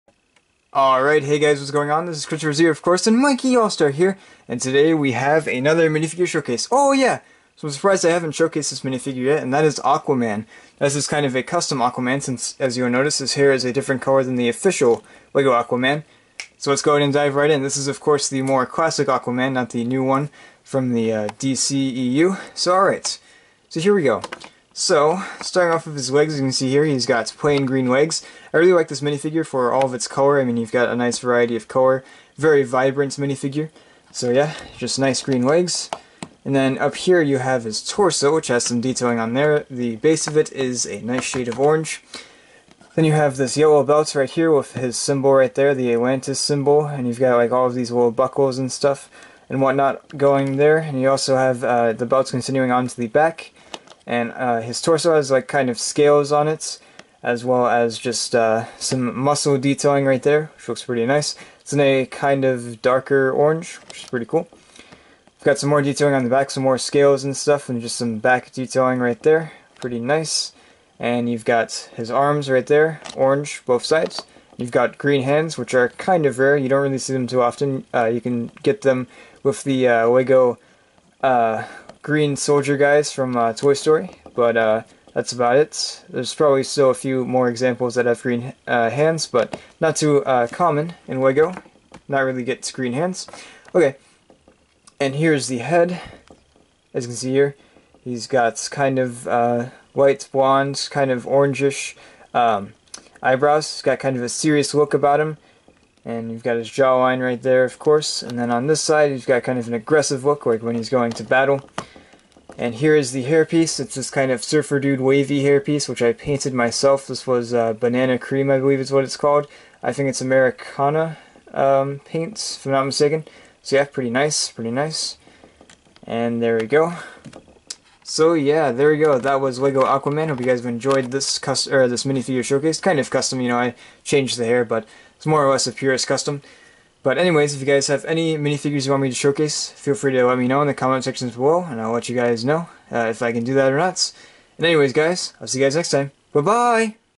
Minifigure Showcase: Aquaman
Welcome back to Minifigure Showcase! Today, we're taking a look at the classic Lego Aquaman minifigure! Enjoy.